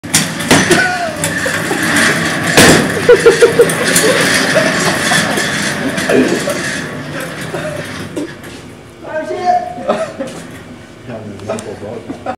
Frank Au Cégep-St-Jérome
Frank Au Cégep-St-Jérome, Durant le tournage du vidéo 3 dans le cours Vidéo 2 dans la technique d'intégration Multimédia